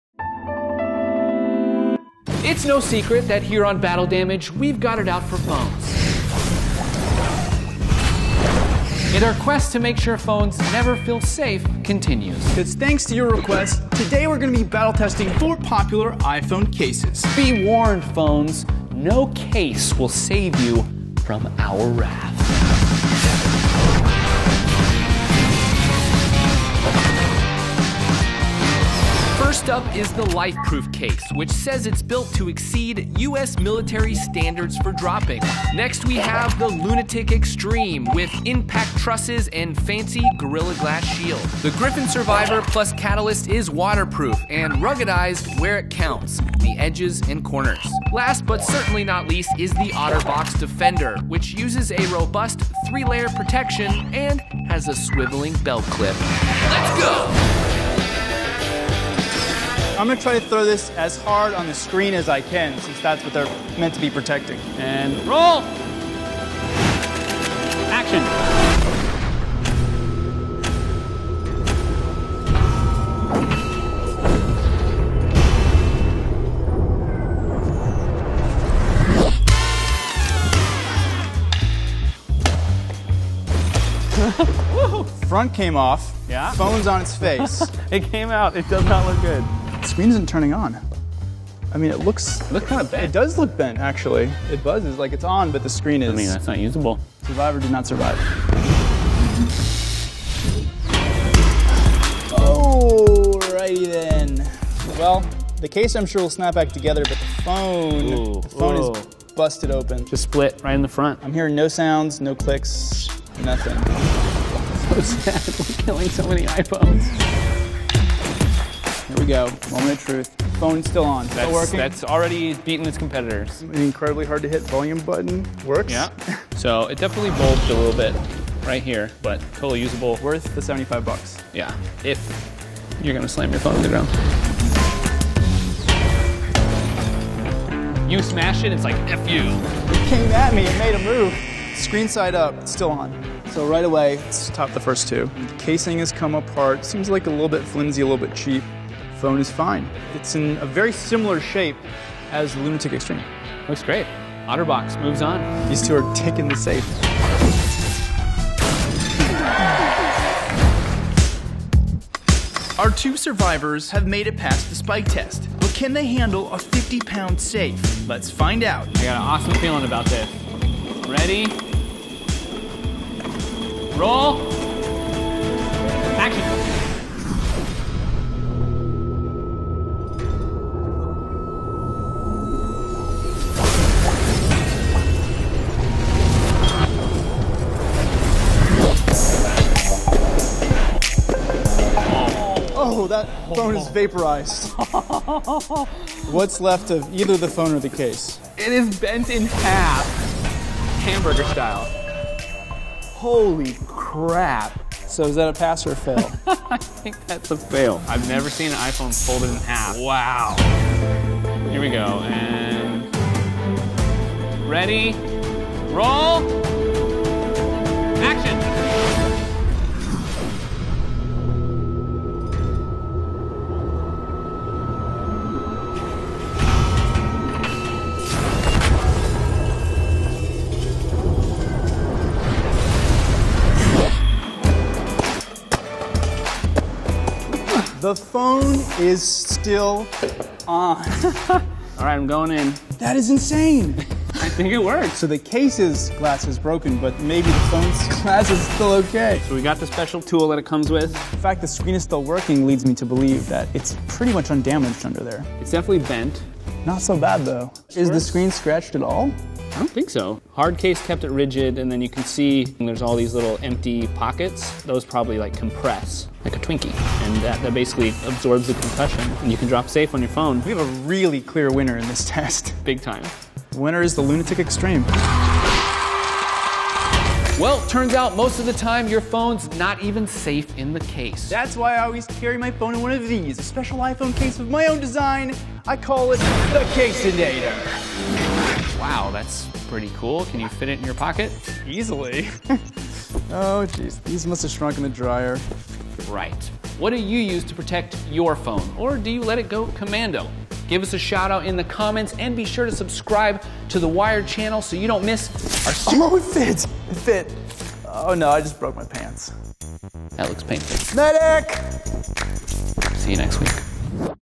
50-pound Safe Folds iPhone in Half | Battle Damage
We stack up four popular iPhone cases, including the OtterBox Defender, the Lunatik TAKTIK Extreme, the Griffin Survivor + Catalyst (note: corrected), and the Lifeproof Fre to see which design will actually keep your smartphone safe.

50-pound Safe Folds iPhone in Half  | Battle Damage

Starring: Justin Johnson and Erik Beck